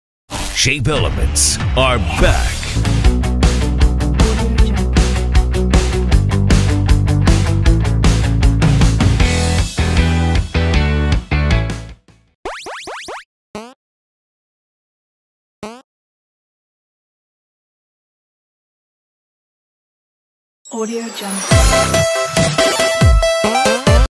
Christmas Pixel | After Effects Template | Openers
Special Thank You ❤ to Maxj13!

Recommended Section:
0:01 Shape Elements 2
2:38 Download 'Dynamic Stomp Opener'
2:38 Download 'Cinematic Opener Slideshow'
2:38 Music
2:46.200 Download 'Subscribe project'
Project does NOT require 3rd party plugins.  and 1 text placeholders.  The price is $22.